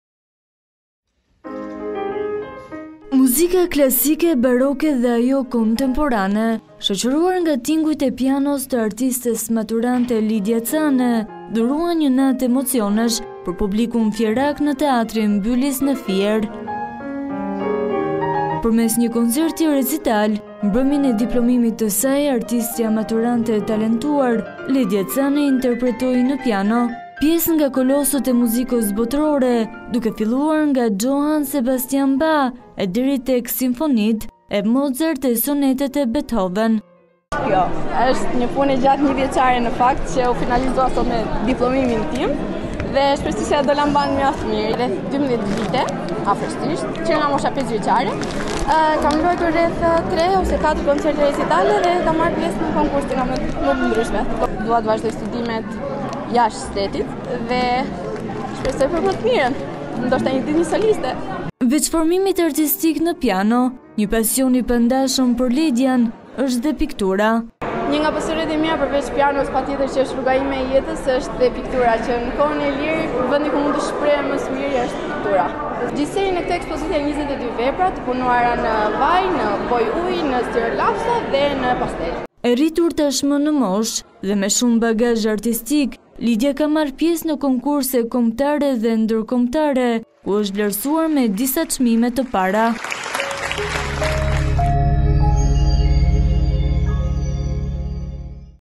Report TV -Studioi piano që 5 vjeç, sot Lidia Cane në koncert recital!
reportTV lajmeshqip lajmePrej moshës 5 vjeçare Lidia Cane ka studiuar piano dhe sot nxënësja e talentuar e shkollës ‘Jakov Xoxa’ në Fier dhuroi emocione në koncertin e saj recital. Ajo rrëfen se sot është e gatshme të konkurrojë në akademitë më prestigjioze të Europës